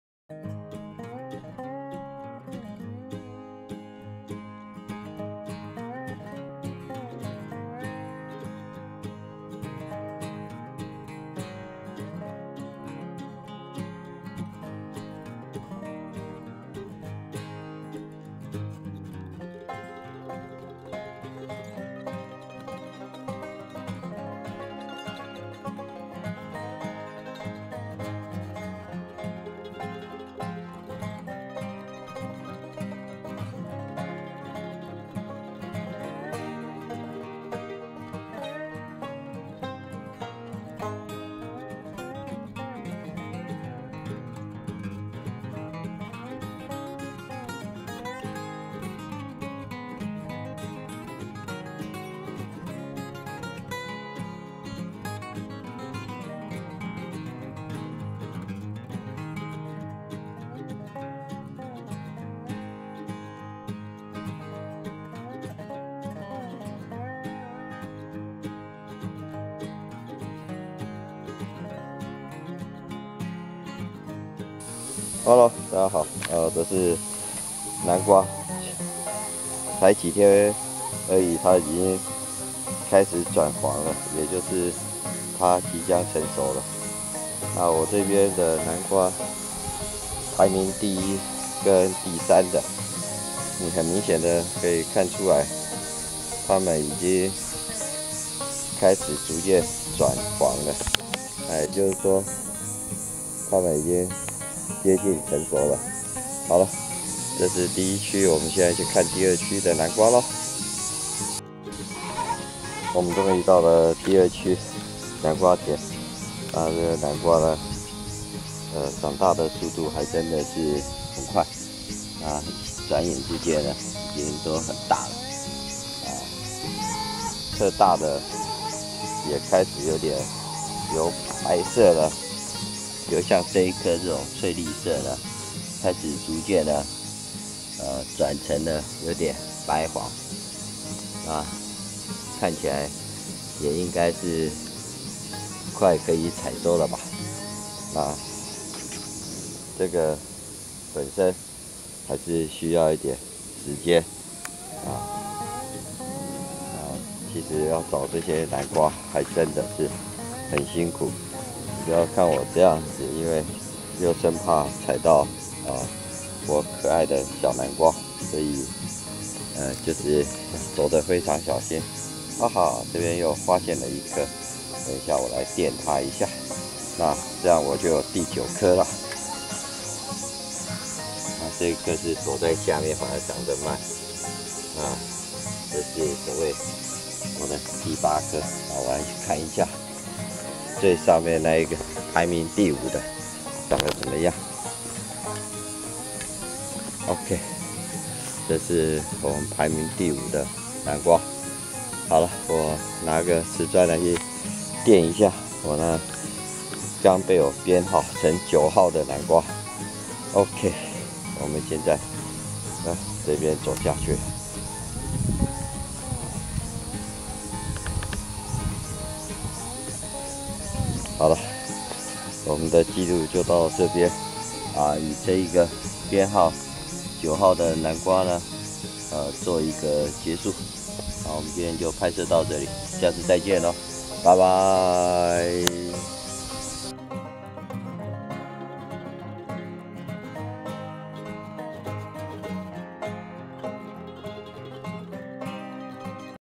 山居伴農－20200616有得有失，農夫的生活寫照
有得有失，農夫的生活寫照
南瓜成長紀錄(5/26  9個)
母雞抱蛋的日子(5/27  四顆蛋)
小黃瓜(6/1  7個)
絲瓜(6/11  4個)
苦瓜(6/14  1個)